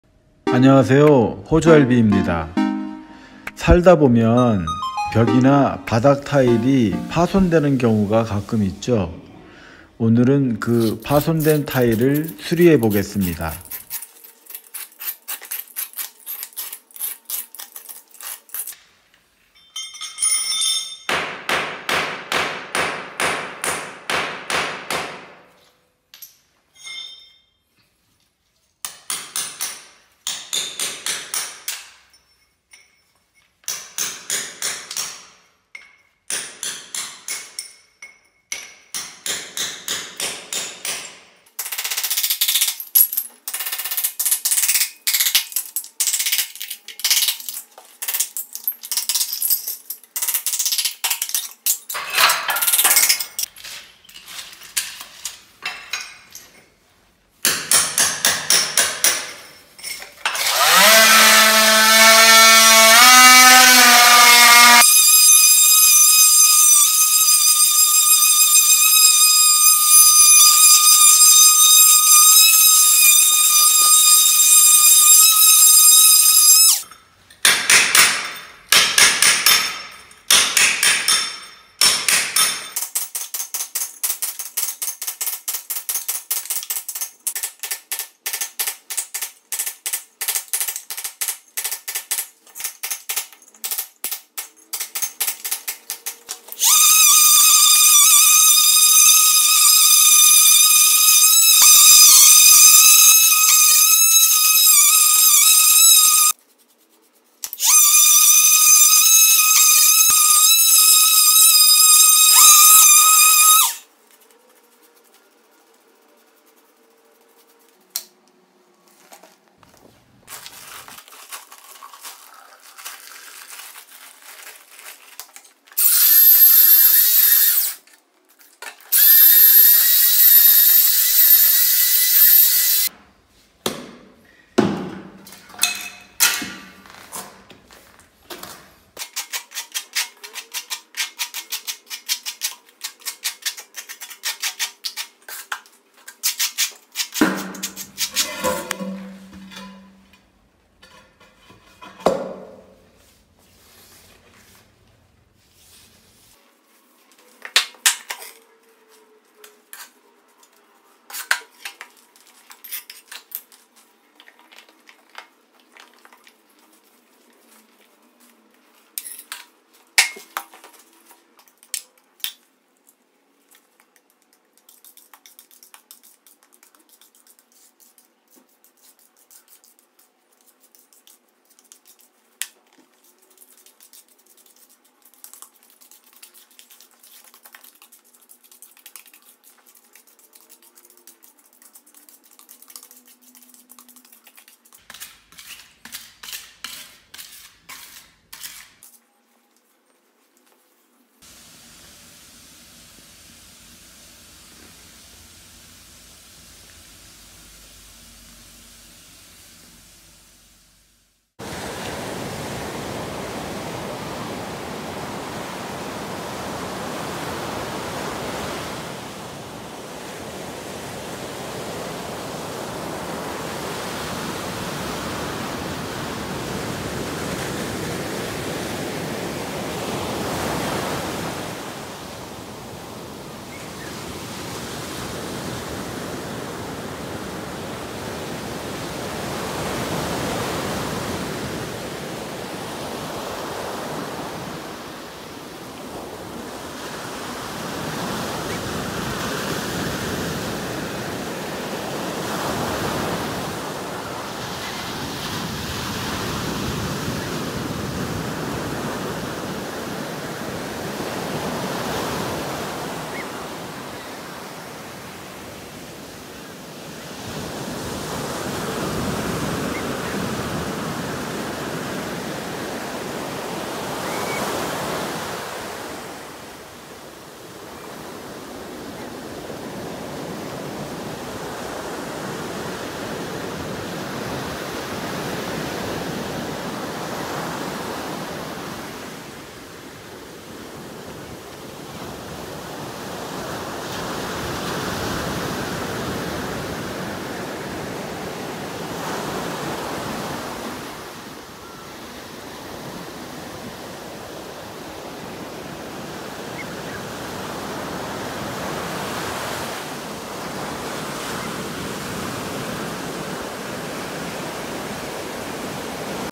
천천히 따라해 보세요 어렵지 않아요. 깨진타일셀프시공.
잘보고 배워서 간단 수리는 셀프시공 하세요.
타일보수 어렵지 않아요.
타일 한장 수리는 기술자도 안해줘요.